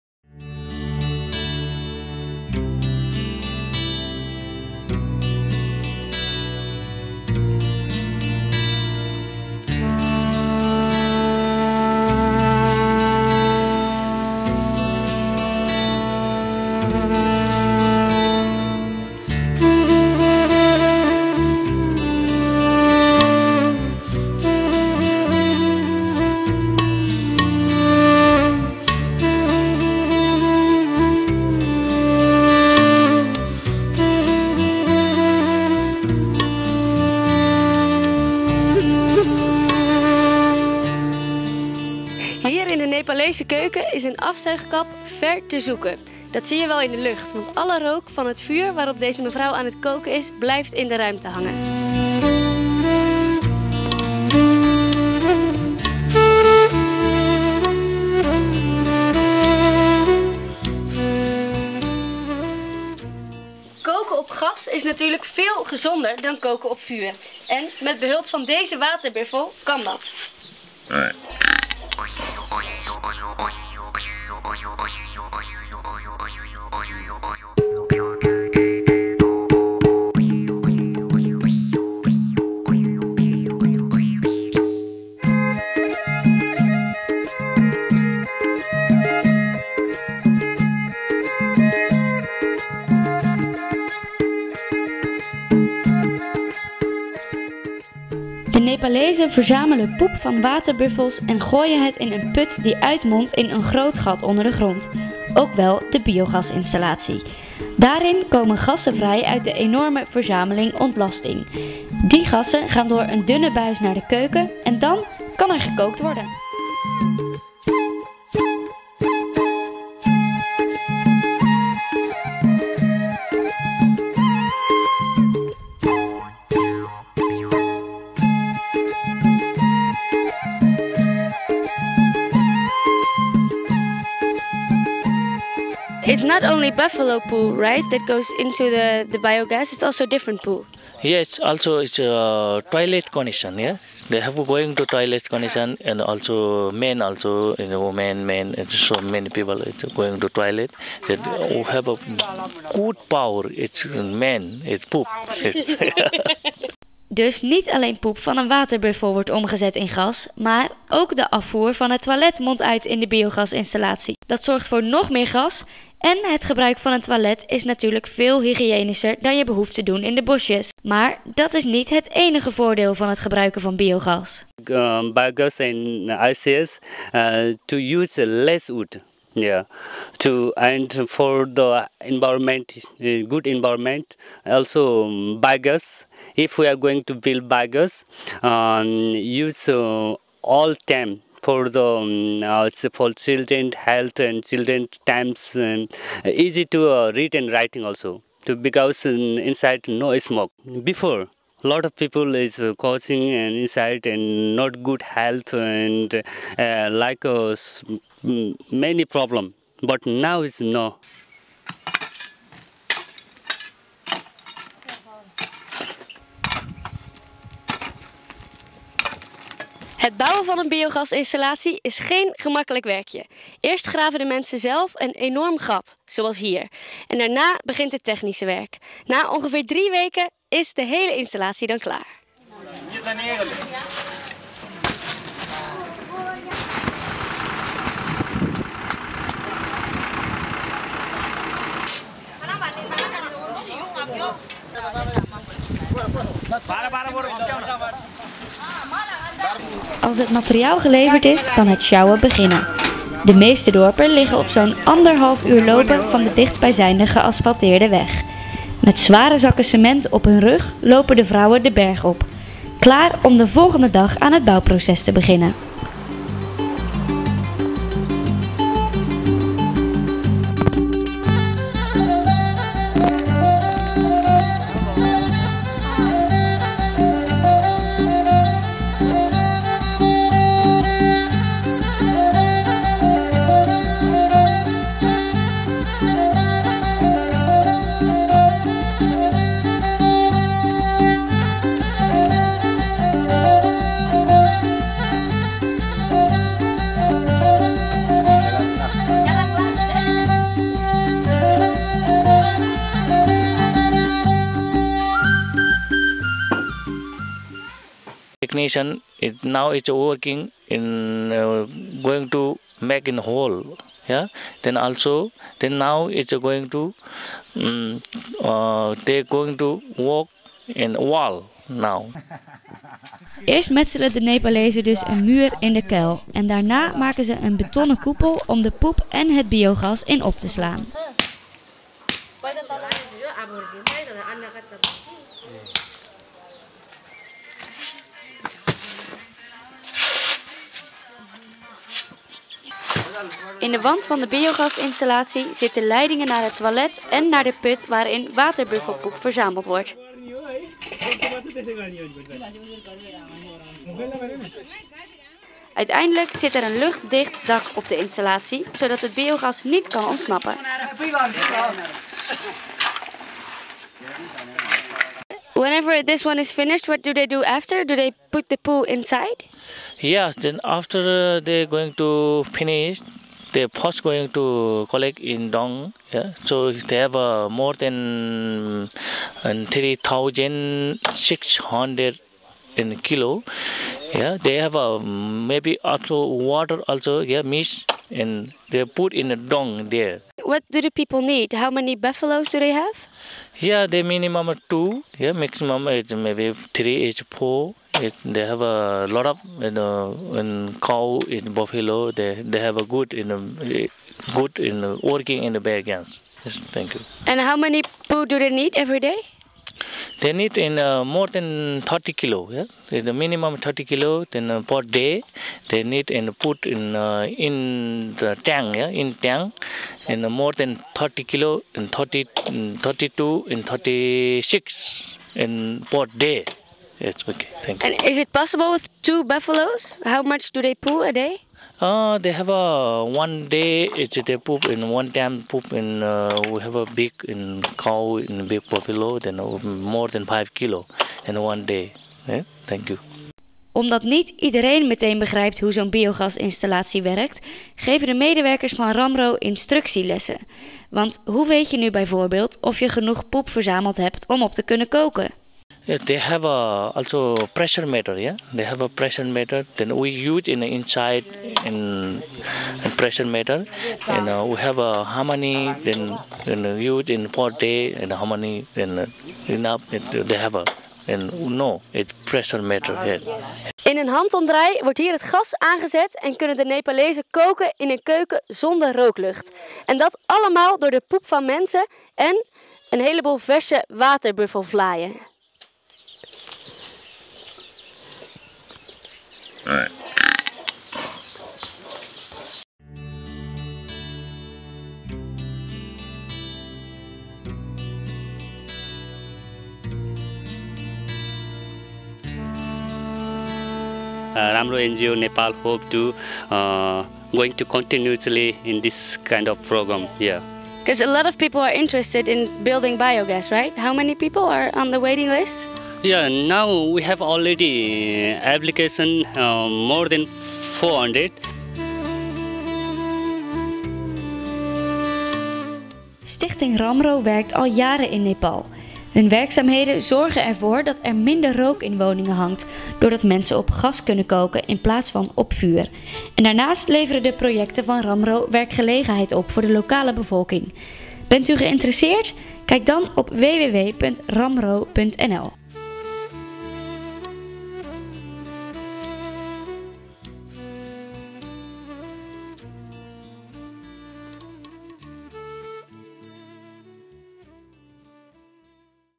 Ramro Biogas
Stichting Ramro realiseert kleinschalige projecten die gericht zijn op structurele en duurzame ontwikkelingssamenwerking in Nepal.

Deze projecten dragen enerzijds bij aan het welzijn van de straatarme Nepalese bevolking door verbetering van de hygiëne, gezondheid, oogst en onderwijs. Anderzijds wordt hiermee een waardevolle bijdrage aan het milieu geleverd, doordat biogasinstallaties een substantiële reductie van CO2 uitstoot realiseren en omdat houtkap, ontbossing en erosie worden beperkt.

Koken op biogas is rookvrij en dat heeft een gunstig effect op de gezondheid. Het spaart het milieu omdat ontbossing en erosie worden verminderd en er is nog een belangrijk effect: het restant van de uitwerpselen dat de biogasinstallatie verlaat bevat geen ziektekiemen meer die bedreigend zijn voor de bewoners (vooral vrouwen en kinderen!). De mest die uit een biogasinstallatie komt - ook wel effluent genoemd - is bovendien een uitstekende voedingsstof voor op het land.